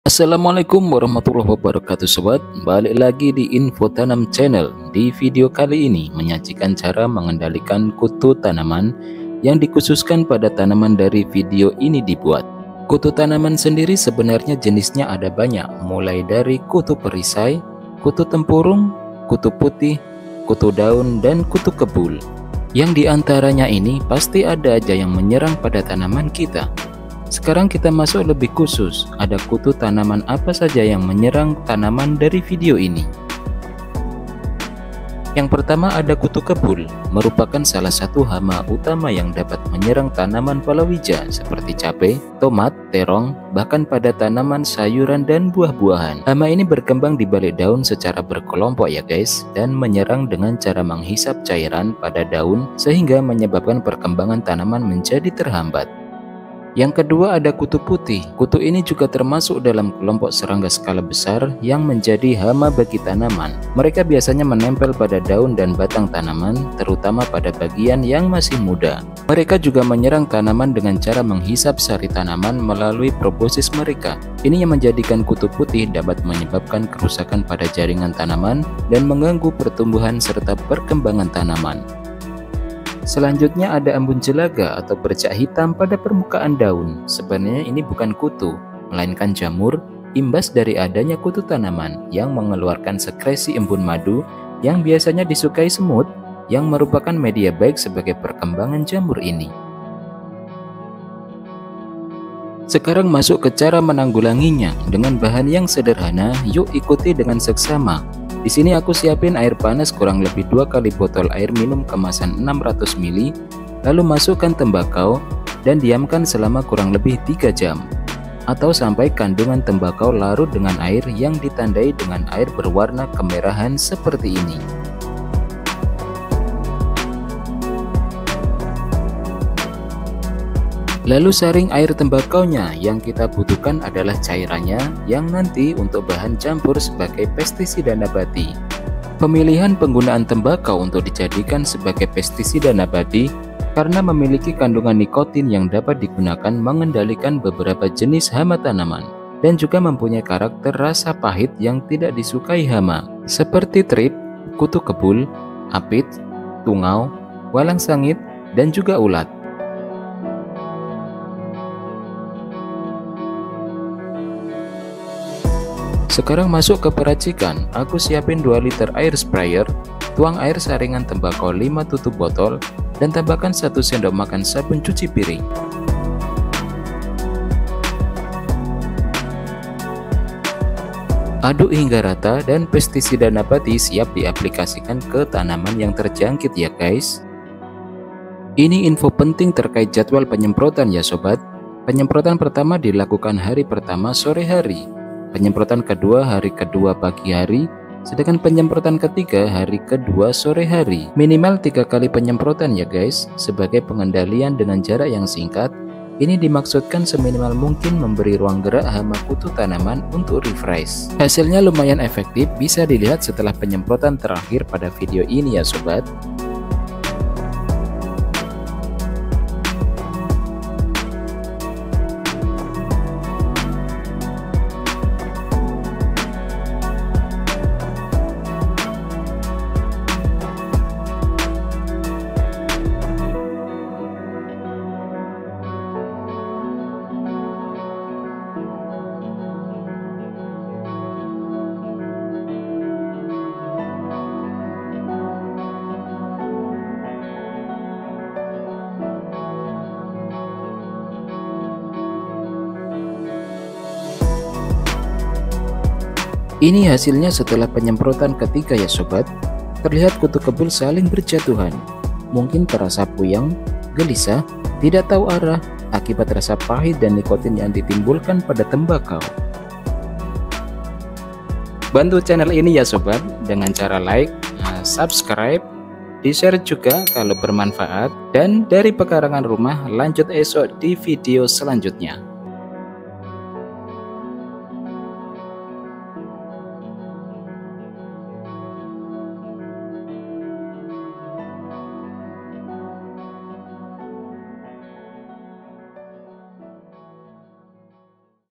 PENGENDALIAN HAMA KUTU TANAMAN DENGAN PESTISIDA NABATI | KUTU SALING BERJATUHAN.
Video ini membahas tentang pengendalian hama yang dikhususkan pada kutu tanaman yang meliputi trips, kutu kebul, kutu putih, kutu daun, tungau dll. dengan menggunakan pestisida nabati, bahan sederhana yang dapat menimbulkan efek tidak sederhana pada hama kutu tanaman. Jadwal penyemprotan efektif juga dibahas di dalam video ini. Tonton videonya hingga akhir ya sobat.

Belajar tanam sayuran di pekarangan rumah, Yukk..penuhi pekaranganmu dengan tanaman.